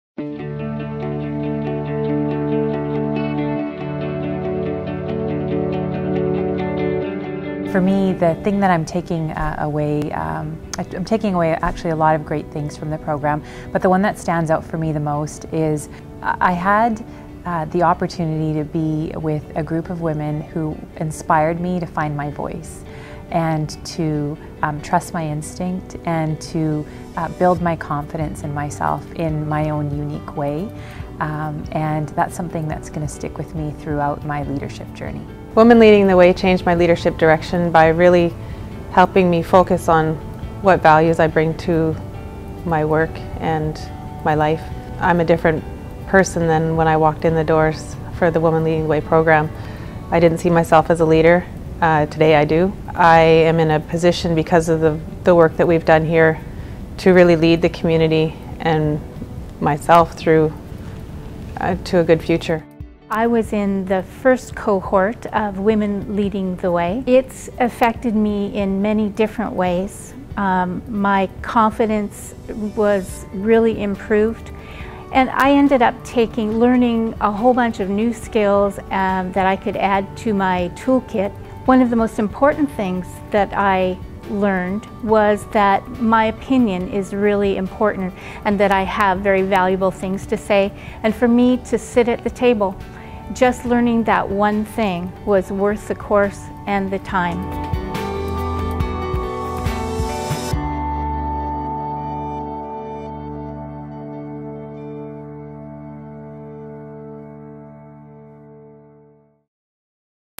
The Minerva Foundation's Women Leading the Way™ Program Testimonials
The Minerva Foundation for BC Women's Women Leading the Way™ (WLW) program is based on the Action Learning Model focused on values-based leadership for advancing leaders. WLW is the third step in the Minerva Foundation's suite of leadership programs and is based on an action-learning and values-based leadership model, which combines classroom learning, reading, research and a hands-on project that will apply learned leadership skills to make a difference in communities across BC.

In addition to gaining information from subject matter experts, participants meet business and community leaders, who will share their experiences and offer their perspective of what makes a successful leader today.